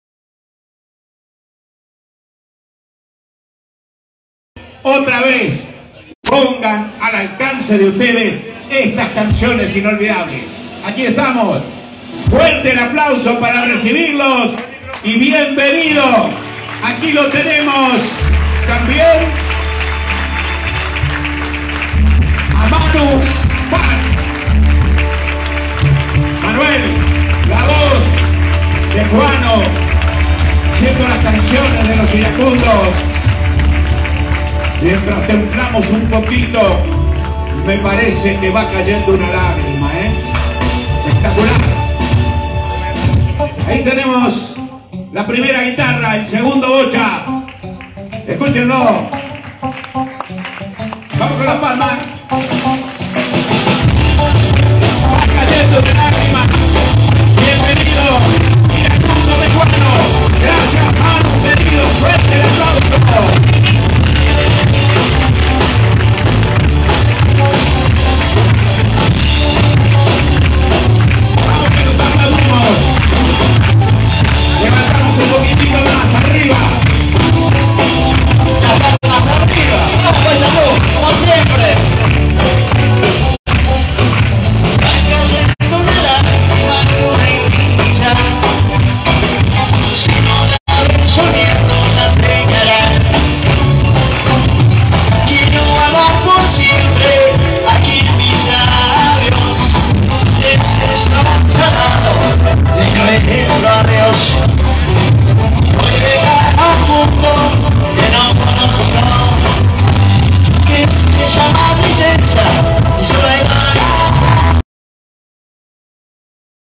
1 de febrero en PAYSANDÚ: presentación de Los Iracundos
HOMENAJE A EDUARDO FRANCO EN PAYSANDÚ: PRESENTACIÓN DE LOS IRACUNDOS POR EL AMIGO FRANCISCO PANCHI COSSO, QUIEN PRESENTÓ EL LIBRO " YO VÍ ACTUAR A LOS IRACUNDOS" .
MÁS INFORMACIÓN EN FACEBOOK:
 ABBA Y LOS IRACUNDOS

GRACIAS A LA INTENDENCIA DE PAYSANDÚ,  SR NICOLÁS OLIVERA EN PARTICULAR POR ÉSTA MARAVILLOSA NOCHE

Gracias a la Dirección de Cultura de Gualeguay por compartir éste momento